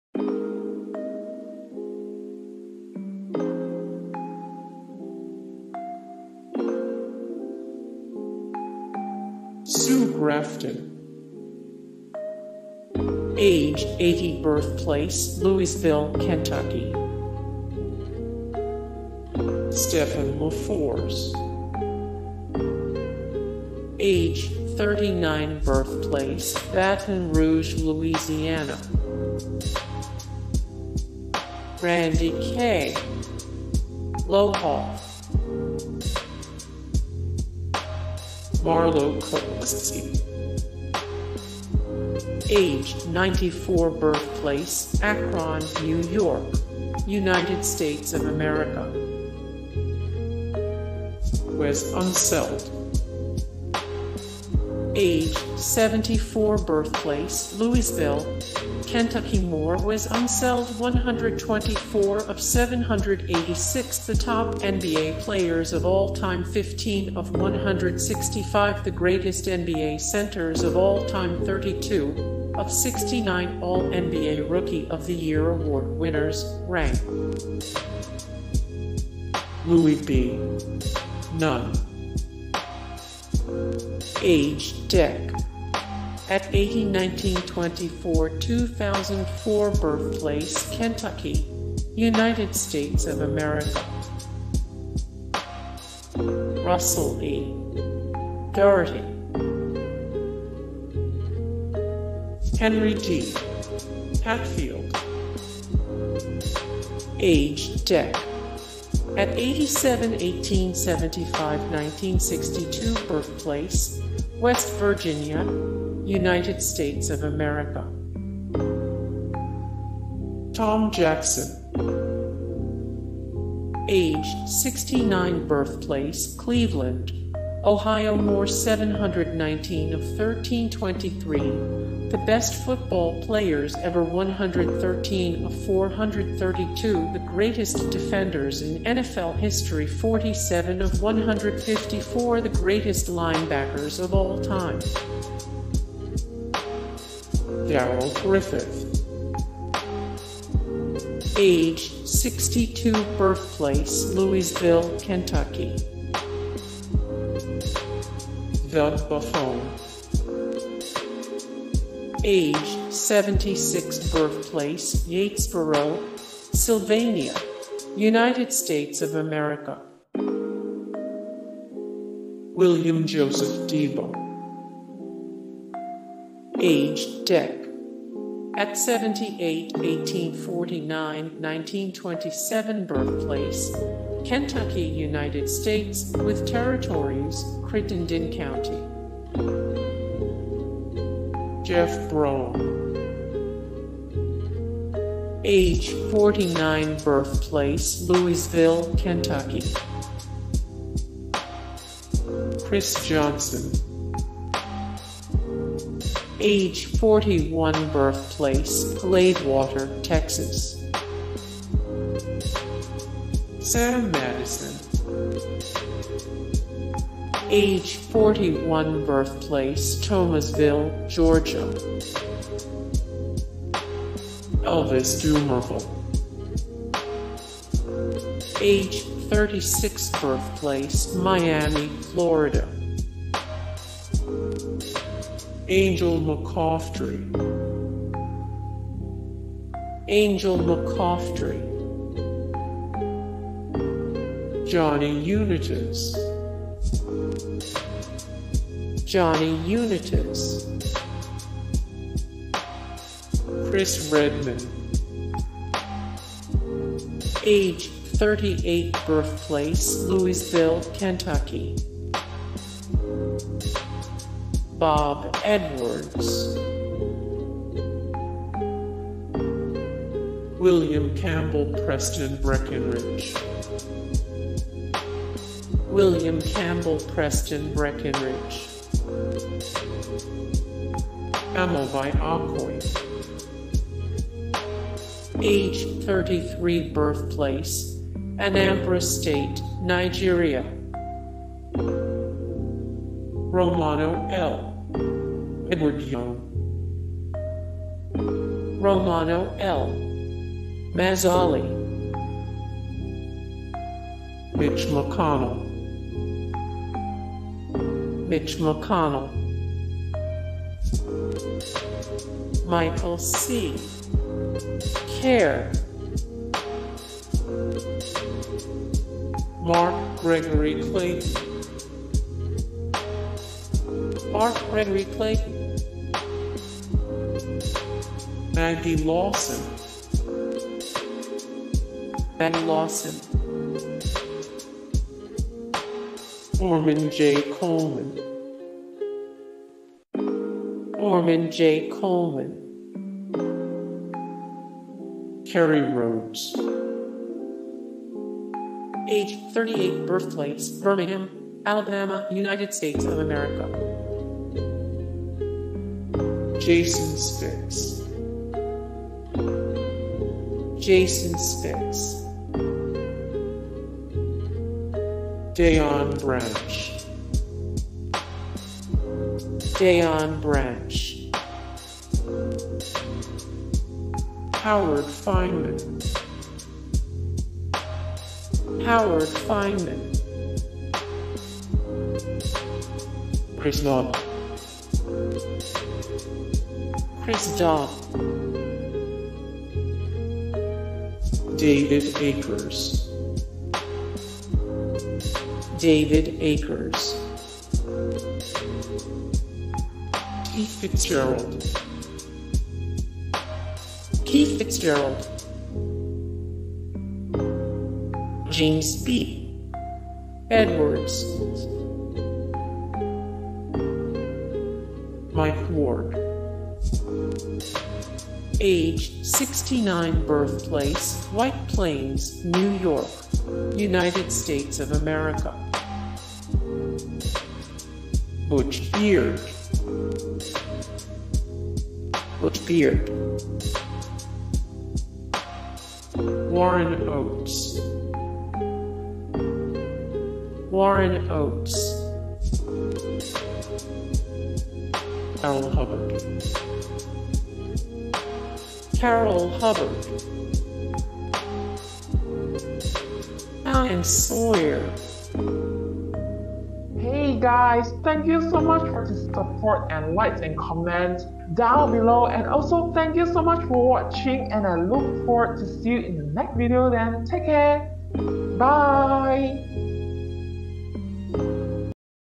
Famous University Of Louisville Alumni ➊
List of famous alumni from University of Louisville, with photos when available. Prominent graduates from University of Louisville include celebrities, politicians, business people, athletes and more. This list of distinguished University of Louisville alumni is loosely ordered by relevance, so the most recognizable celebrities who attended University of Louisville are at the top of the list. This directory is not just composed of graduates of this school, as some of the famous people on this list didn't necessarily earn a degree from University of Louisville.  A list made up of graduates like Johnny Unitas and Wes Unseld. This list answers the questions Which famous people went to University of Louisville? and Which celebrities are University of Louisville alumni?...more
0:00 - Intro
0:00:08 - Sue Grafton
0:00:19 - Stefan LeFors
0:00:32 - Randy K. Lohoff
0:00:40 - Marlow Cook
0:00:54 - Wes Unseld
0:01:23 - Louie B. Nunn
0:01:41 - Russell E. Dougherty
0:01:48 - Henry D. Hatfield
0:02:07 - Tom Jackson
0:02:35 - Darrell Griffith
0:02:47 - Doug Buffone
0:03:02 - William Joseph Deboe
0:03:22 - Jeff Brohm
0:03:34 - Chris Johnson
0:03:46 - Sam Madison
0:03:58 - Elvis Dumervil
0:04:10 - Angel McCoughtry
0:04:20 - Johnny Unitas
0:04:30 - Chris Redman
0:04:42 - Bob Edwards
0:04:49 - William Campbell Preston Breckinridge
0:05:01 - Amobi Okoye
0:05:14 - Romano L. Mazzoli
0:05:26 - Mitch McConnell
0:05:35 - Michael C. Kerr
0:05:43 - Mark Gregory Clayton
0:05:53 - Maggie Lawson
0:06:02 - Norman Jay Coleman
0:06:12 - Kerry Rhodes
0:06:24 - Jason Spitz
0:06:34 - Deion Branch
0:06:44 - Howard Fineman
0:06:54 - Chris Dodd
0:07:03 - David Akers
0:07:13 - Keith Fitzgerald
0:07:22 - James B. Edwards
0:07:30 - Mike Ward
0:07:44 - Butch Beard
0:07:53 - Warren Oates
0:08:03 - Carroll Hubbard
0:08:12 - Diane Sawyer